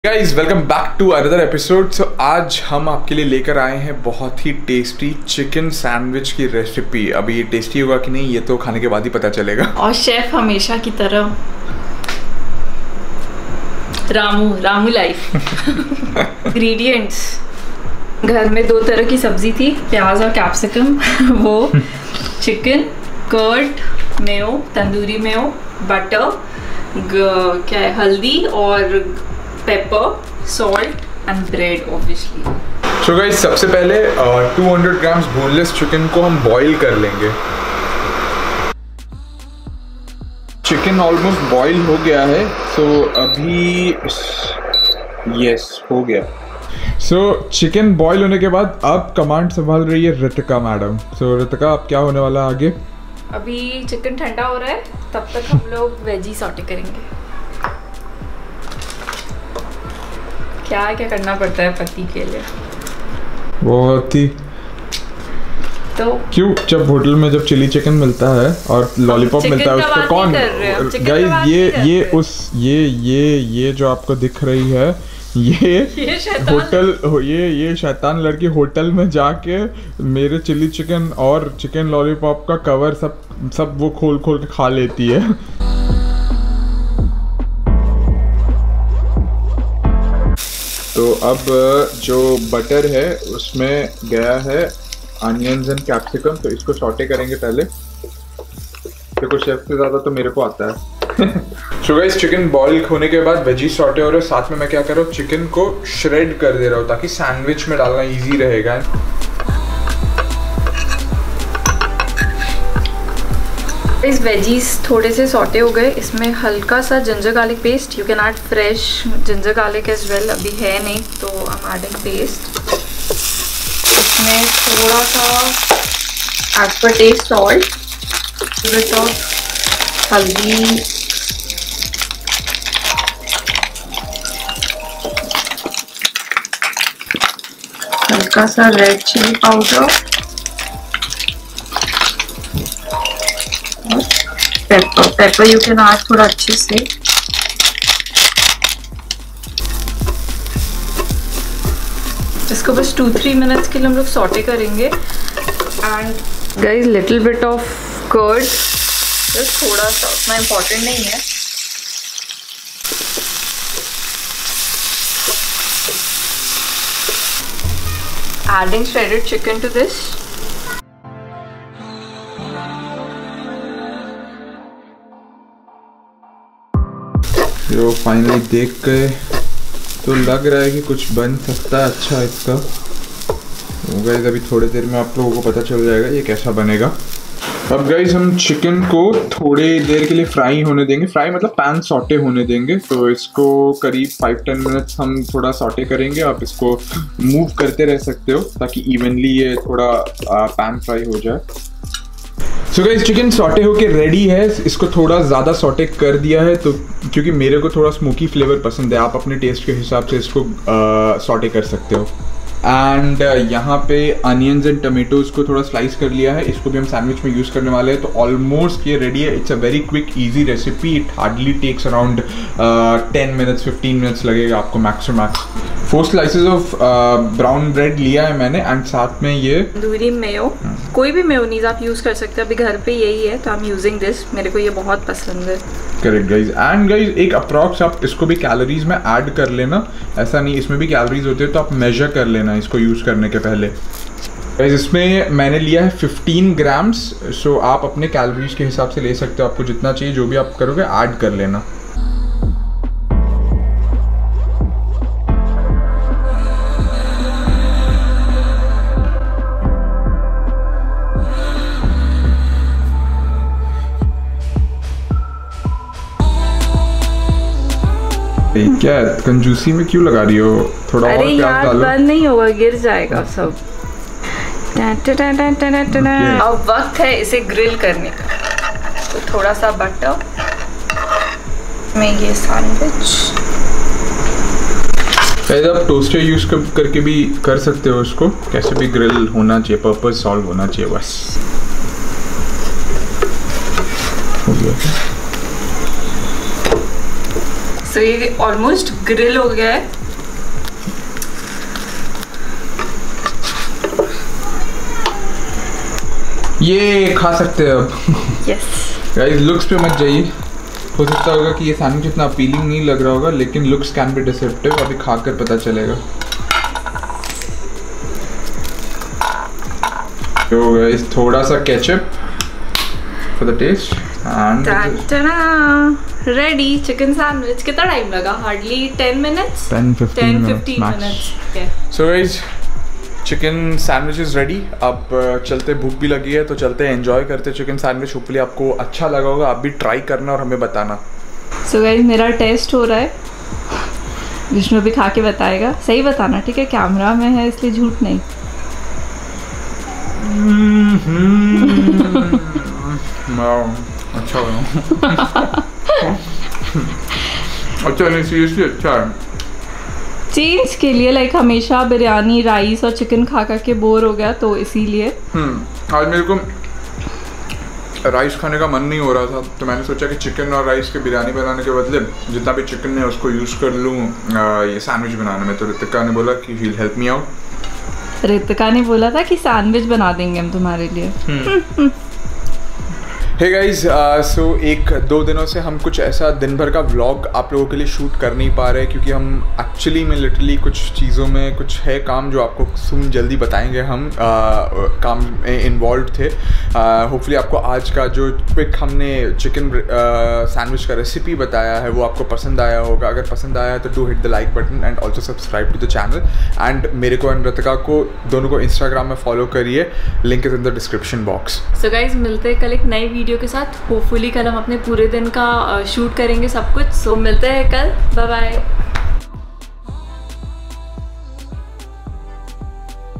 Quick Easy Chicken Recipe | High Protein
Quick easy chicken recipe who wants to try something tasty and healthy and  wants it real quick within 15 minutes.

Rode Mic

Joby gorillapod

Jmary tripod

Fur Jaden Bag

Memory Card Case

Memory Card